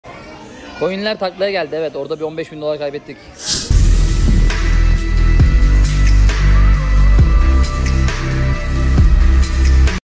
LAPTOPLUGEZGİN COİNLERDEN NE KADAR ZARAR ETTİĞİNİ AÇIKLIYOR/laptoplugezgin TWİTCH kesitleri #13
Canlı yayınları kaçırmamak için Twitch.tv/laptoplugezgin takip edebilirsiniz